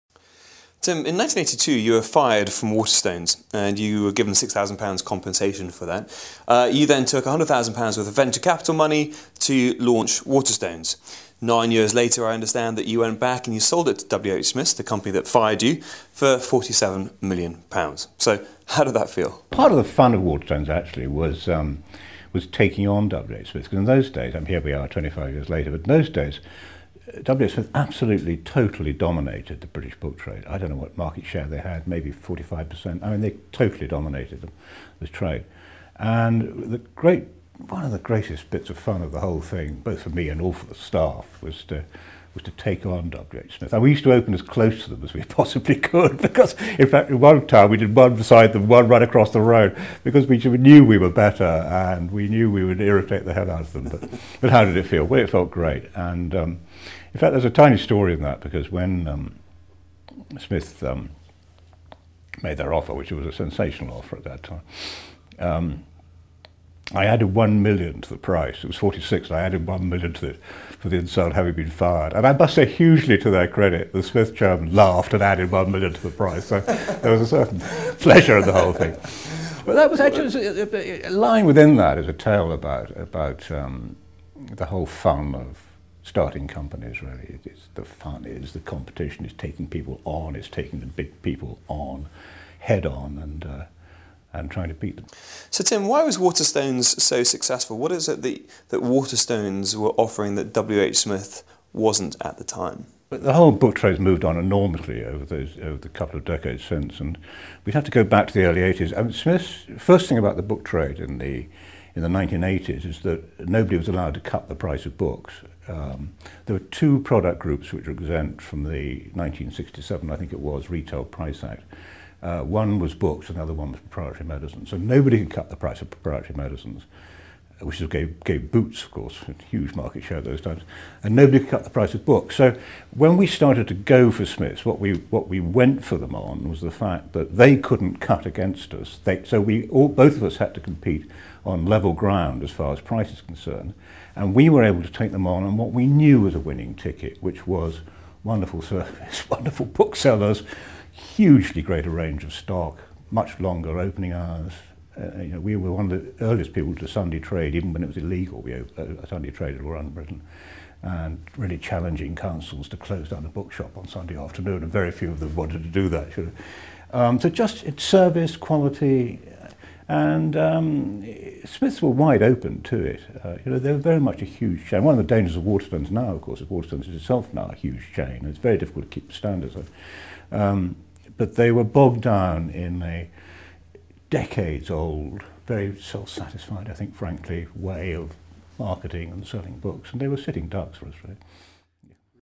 Taking on The Competition
B Vo The Business Voice - world leaders at your desk brought to you

The former Chairman of HMV Media Group and Founder of Waterstone's retailers, Tim is one of the most astute and determined businessmen of the last decade.

Tim founded the remarkably successful Waterstone's booksellers with £6000 of the redundancy money he received when he was sacked from WH Smith in 1982.  Nine years later he sold the company to his previous employers for £47m!  We think that's pretty cool.  Learn how he did it and how important it it to have a competitive strategy!